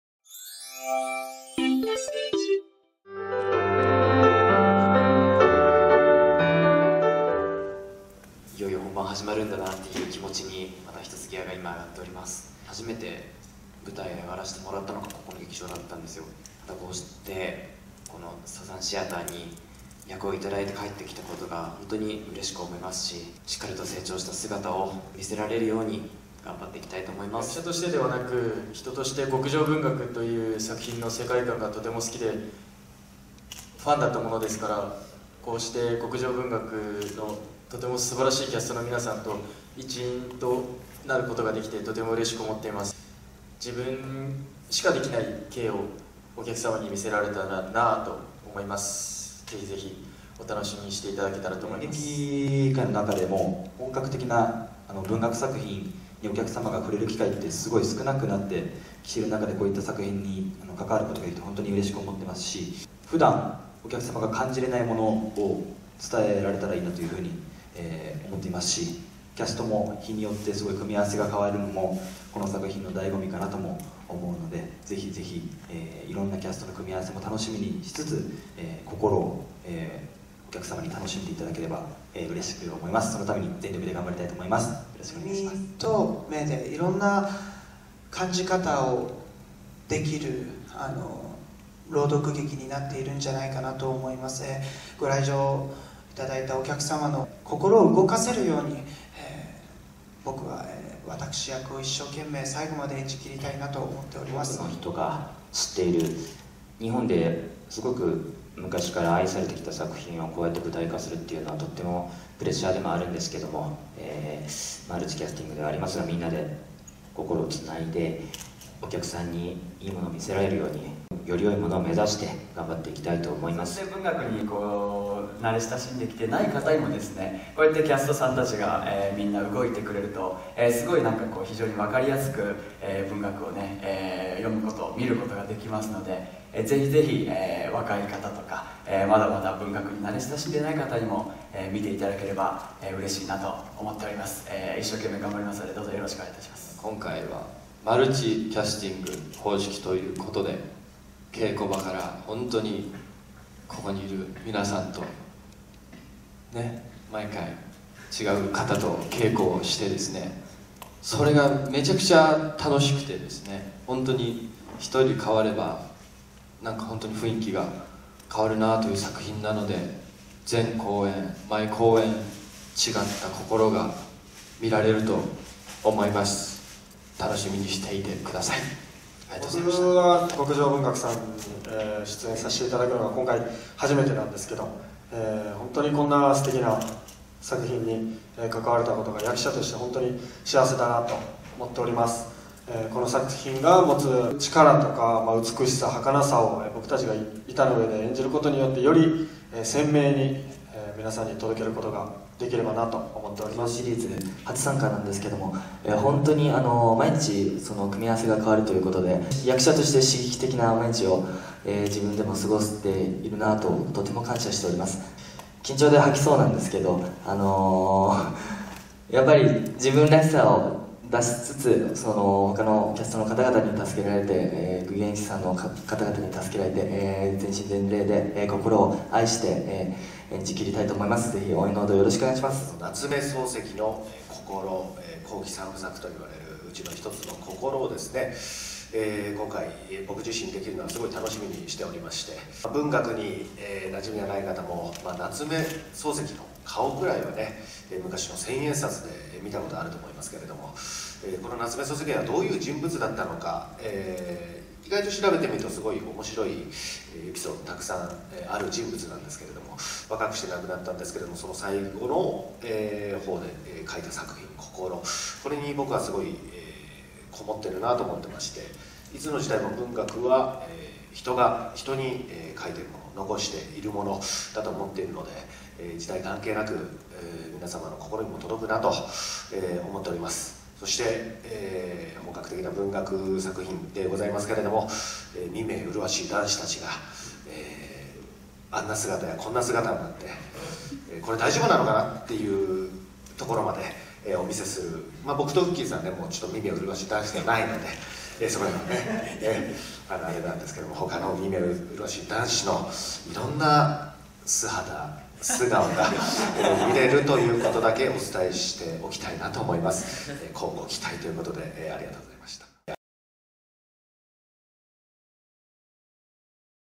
極上文學第13弾『こゝろ』囲み会見 | エンタステージ
2018年12月13日（木）に東京・紀伊國屋サザンシアター TAKASHIMAYAにて極上文學の第13弾公演『こゝろ』が開幕した。「読み師」であるメインキャストの周りを「具現師」が 舞い、「語り師」が彩りを添える極上文學に、ついに、夏目漱石の不朽の名作「こゝろ」が登場。開幕を前に、「読み師」より、”私（わたくし）”役の櫻井圭登、内海啓貴、K役の芹沢尚哉、松井勇歩（劇団 Patch）、釣本南（Candy Boy）、妻（さい）役の白石康介、東拓海、先生役の藤原祐規、平野良、そして「語り師」より沢城千春が登壇し、意気込みを語りました。

◆公演情報
本格文學朗読演劇シリーズ 「極上文學」第13 弾『こゝろ』
2018年12月13日（木）～12月18日（火）　東京・紀伊國屋サザンシアター TAKASHIMAYA
【原作】夏目漱石
【演出】キムラ真（ナイスコンプレックス）
【脚本】神楽澤小虎（MAG.net）
【音楽】橋本啓一

【出演】
「読み師」私：櫻井圭登、内海啓貴 K：芹沢尚哉、松井勇歩(（劇団Patch）)、釣本南（Candy Boy）妻：白石康介、東拓海 先生：藤原祐規／平野良
「具現師」福島悠介、百瀬友水、毛利光汰、小野田唯、古賀雄大
「語り師」伊東健斗、駒田航、沢城千春、寺島惇太、西山宏太朗、野上翔、村田太志 （五十音）

(C)2018 CLIE/MAG.net